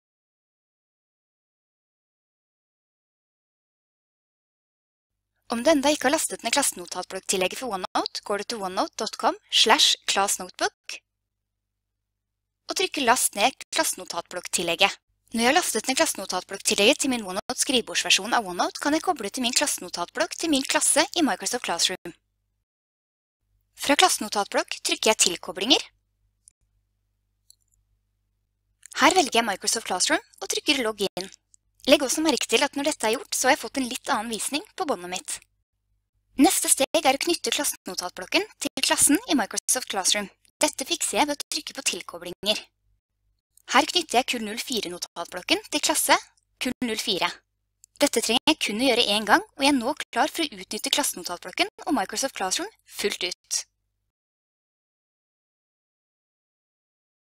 Koble OneNote Klassenotatblokk til Microsoft Classroom
Dette er modul 7 av @steffisteaching Microsoft Classroom.

Dette er en tredelt modul som tar for seg hvordan du kan spare tid ved å koble OneNote klassenotatblokken din til Microsoft Classroom. I denne modulen ser vi på hvordan vi kobler en OneNote klassenotatblokk til Microsoft Classroom.